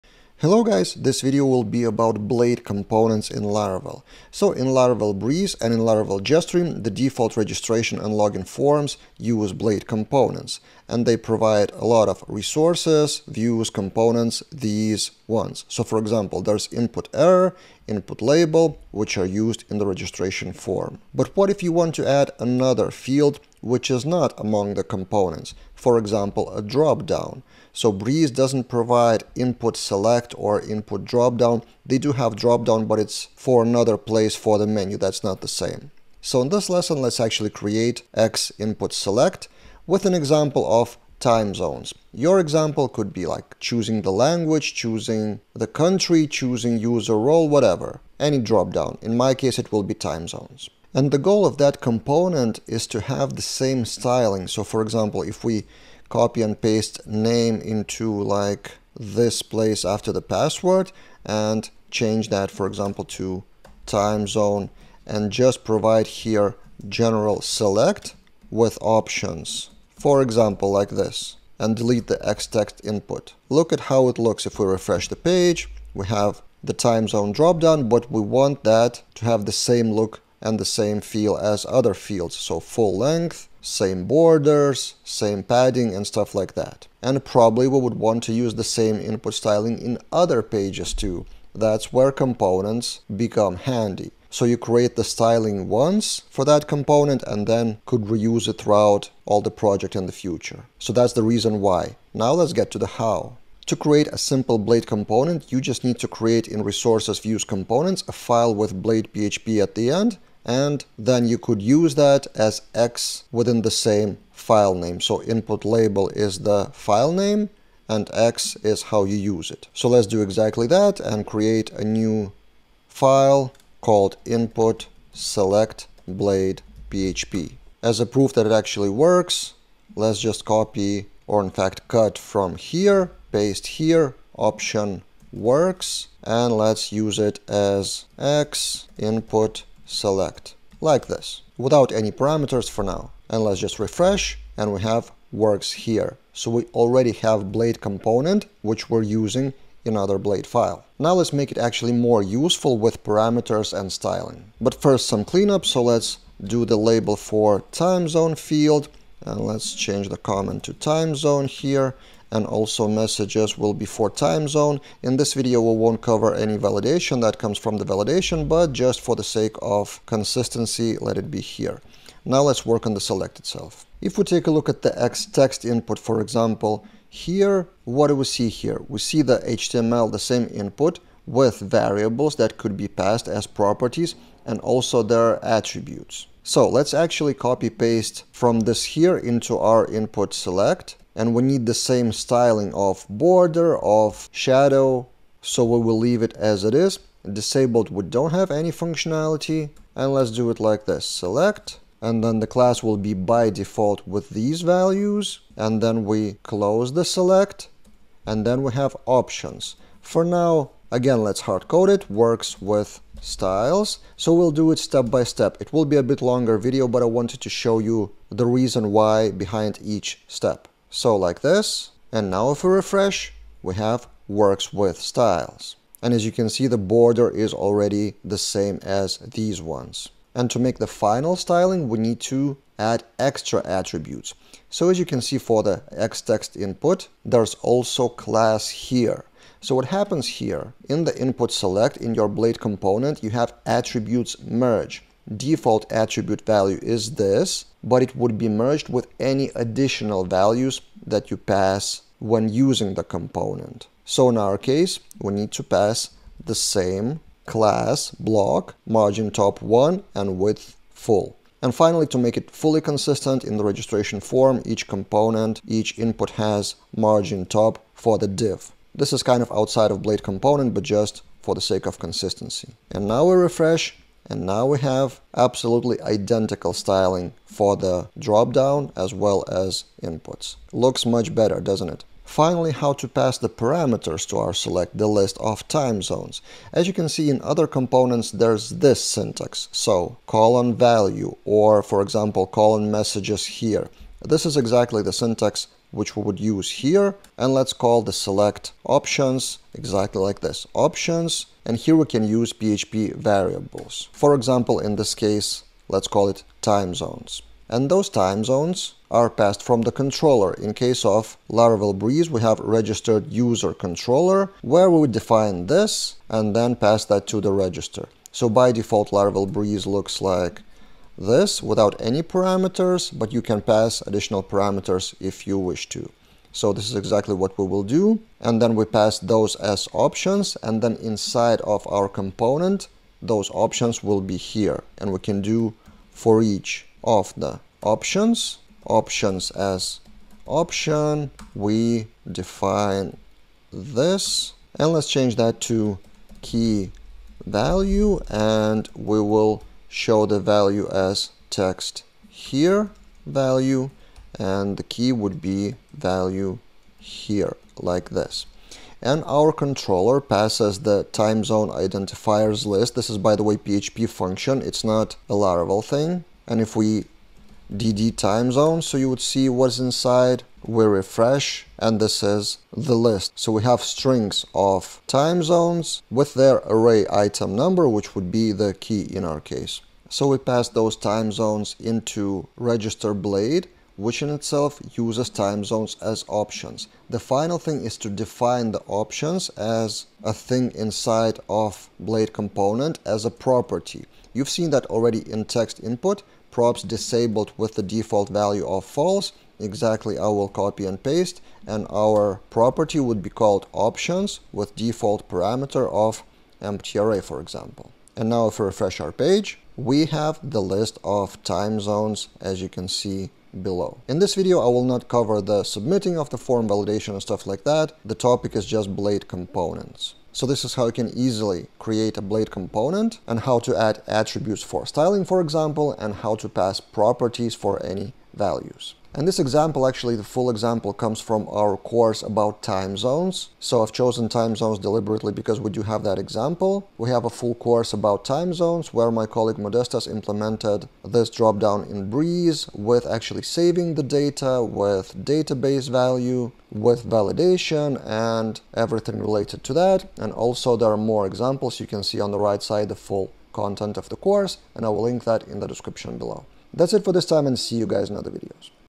Creating Laravel Blade Component: Step-By-Step
Let's take an example Blade component x-text-input from Laravel Breeze and create a new similar one x-input-select.